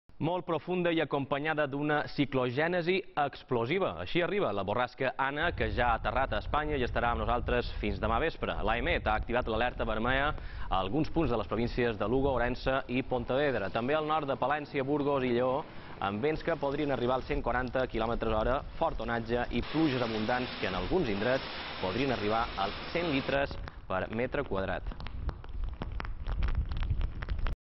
La borrasca Ana ja afecta Espanya
La borrasca ANA que ja ha arrbiat a Espanya de forma molt profunda i acompanyada d’una ciclogènesi explosiva. Afectarà el país fins demà vespre. L’AEMET ha activat l’alerta vermella a alguns punts de les províncies de Lugo, Ourense i Pontevedra. També al nord de Palència, Burgos i Lleó amb vents que podrien arribar als 140 quilòmetres hora, fort onatge i pluges abundants que en alguns indrets podrien arribar als 100 litres per metre quadrat.